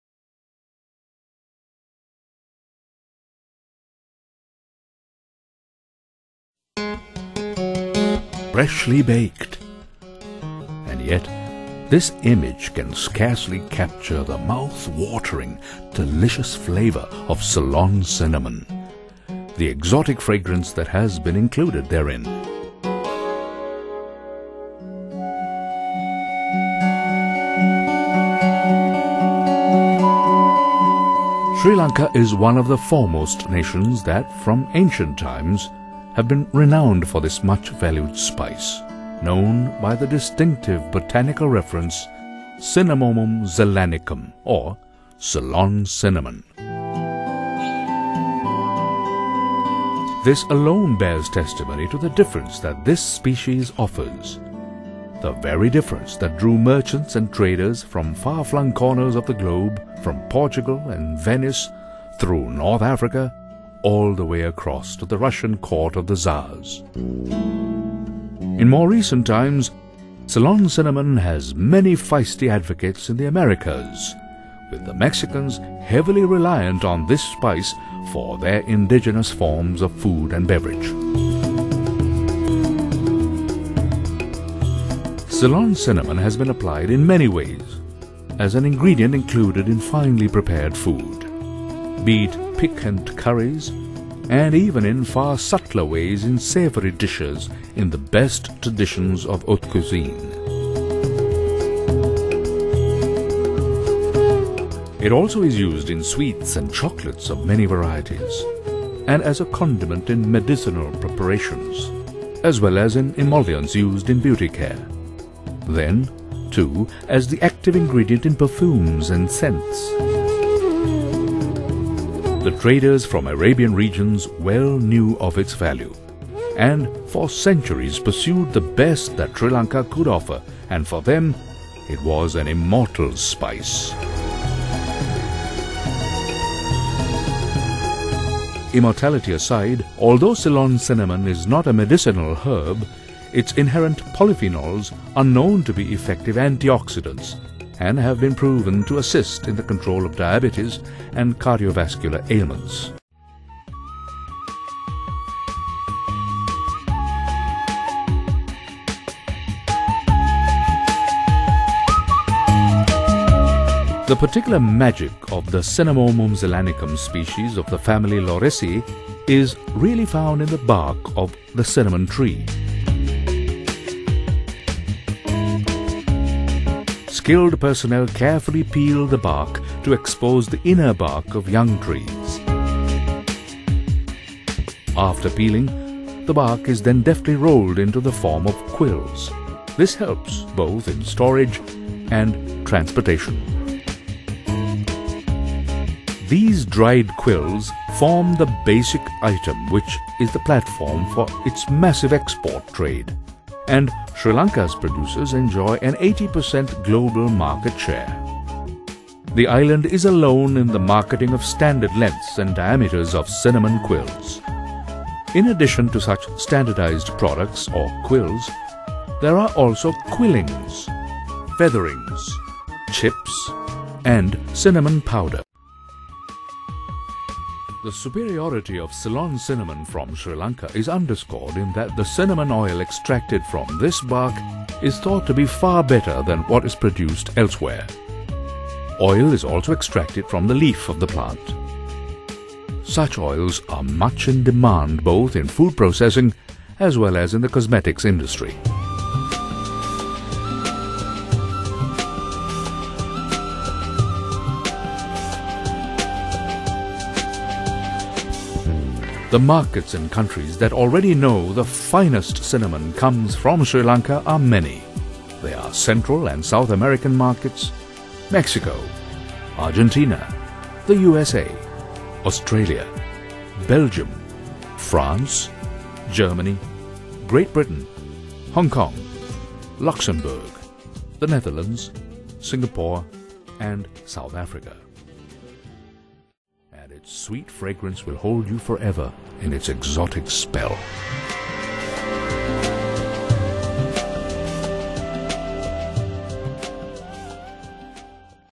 Indus Organics Ceylon Cinnamon From Sri Lanka
Indus Organics presents Ceylon Cinnamon. This movie shows the history, usage, medicinal benefits and production of premium Ceylon Cinnamon from Sri Lanka.